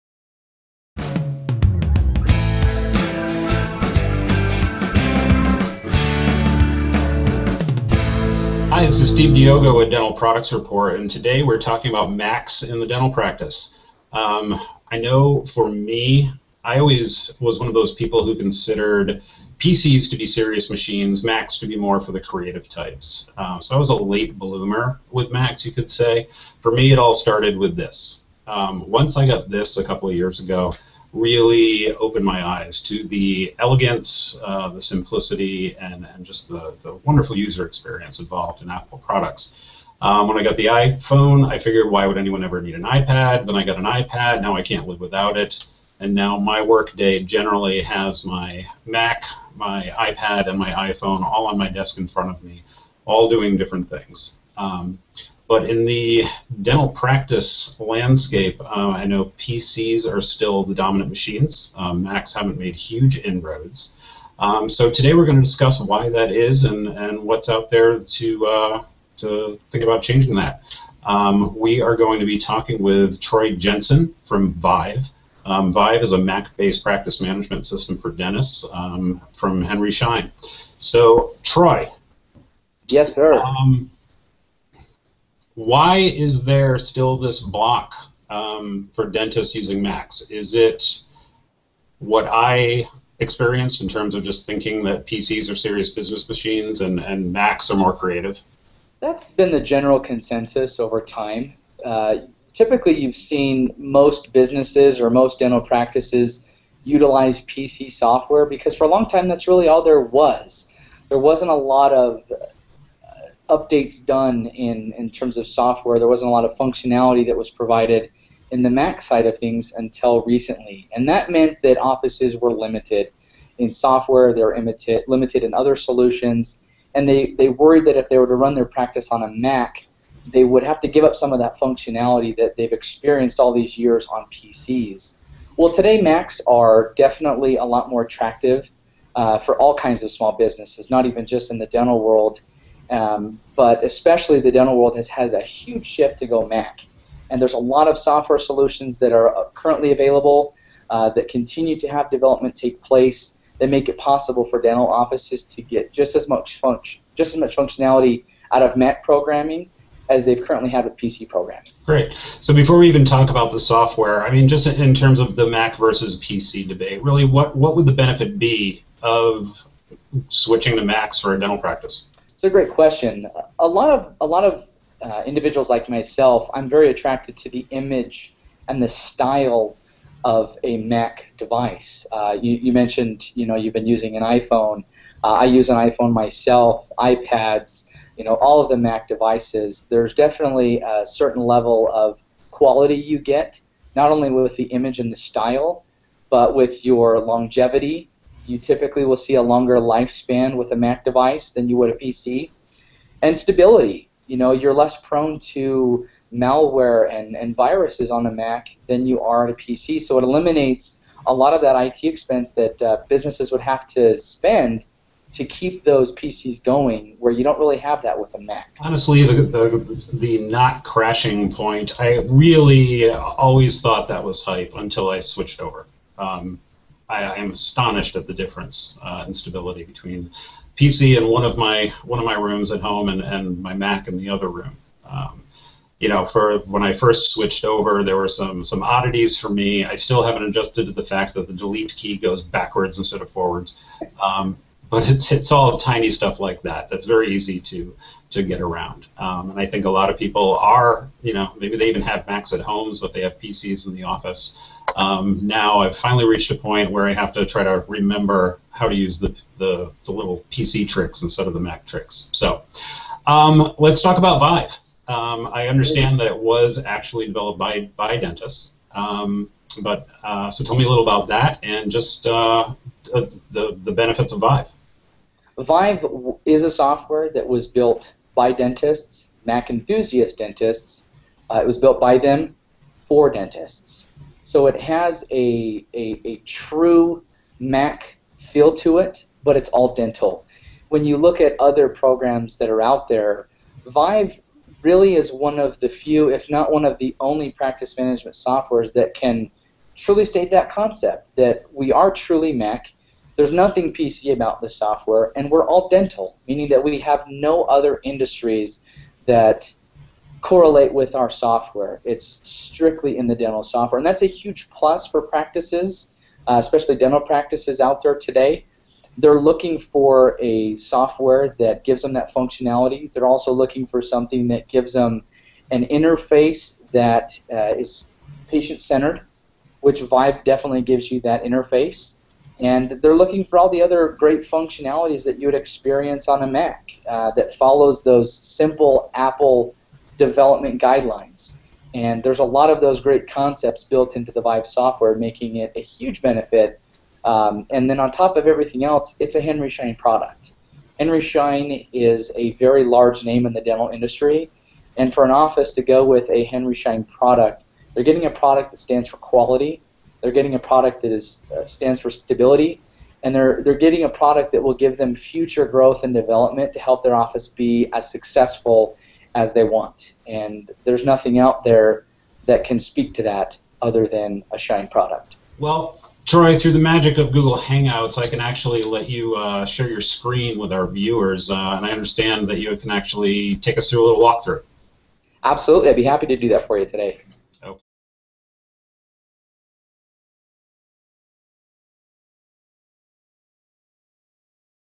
Learn How to Use Macs in the Dental Practice with Viive Software
Learn the benefits of Macs in the dental practice and how to integrate Apple products using the Viive Dental Practice Management Software System from Henry Schein. For a demonstration of Viive Mac Dental Software, watch